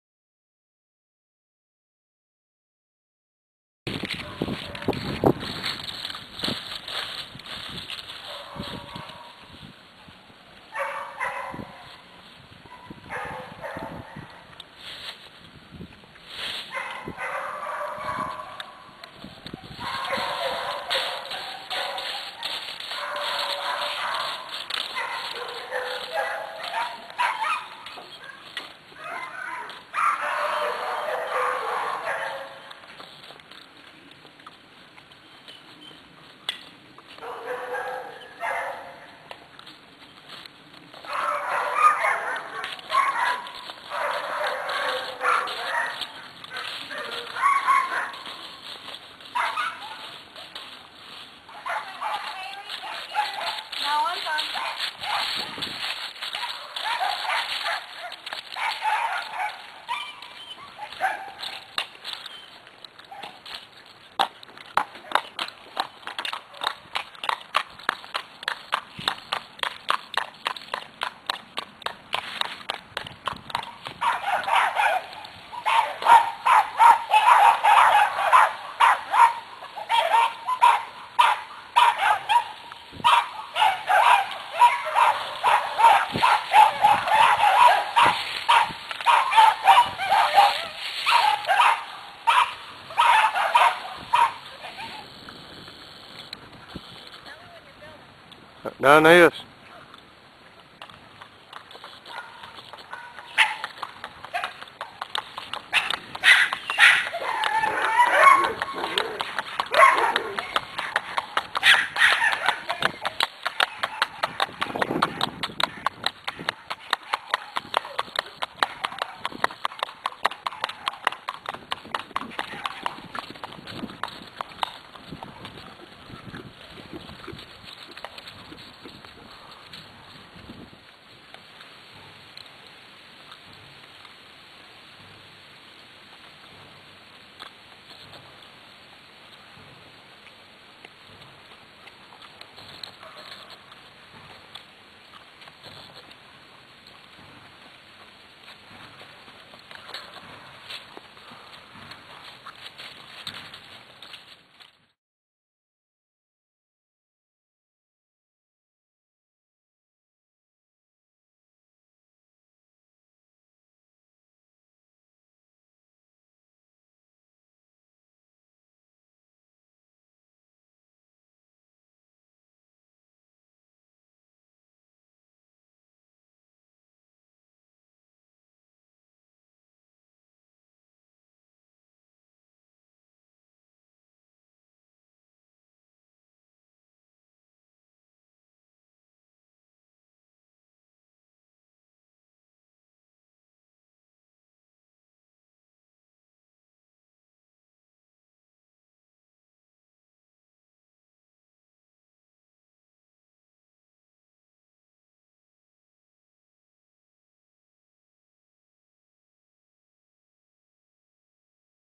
Rebels dangerous diana
This is Rebel's Dangerous Diana. She is a coming 3 yr old broke easy going mare. She has a coming white mane and tail and has a great  natrual gait. She also is  the 2009 UCWHA high point champion in the 2-3 yr old park pleasure division. She has been showing this past year in country pleasure with a keg shoe and in park pleasure with a plantation shoe. She is sired by the great Gens armed and dangerous out of a harlinsdale mare by Dark spirits Rebel. This horse is by far one of the best mares i have had. She is so pretty and sweet. Goes anywhere you point her and has had kids ride her on trails and shows. That says alot about her right there.